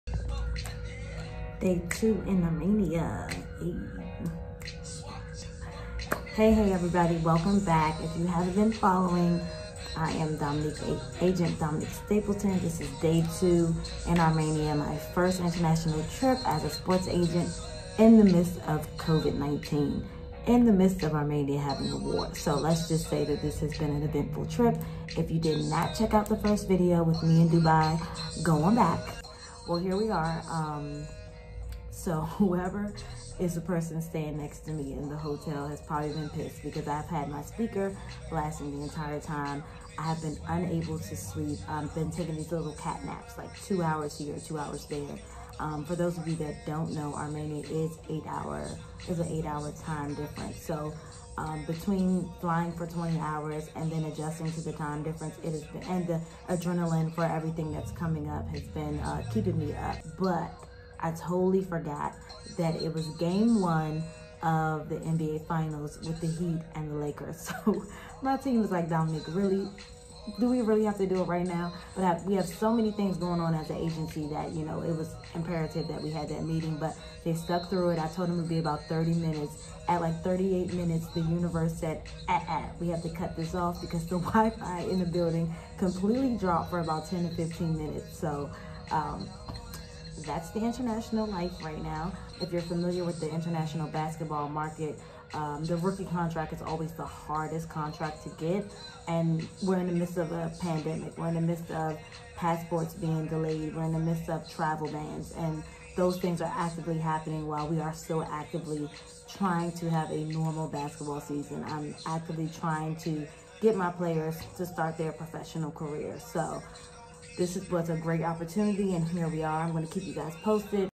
Eurobasket Armenian Basketball Tour As A Sports Agent
Agent Dominique Stapleton attends Eurobasket Armenian Basketball Tour 2020. Day one in Armenia.

Are we connected socially?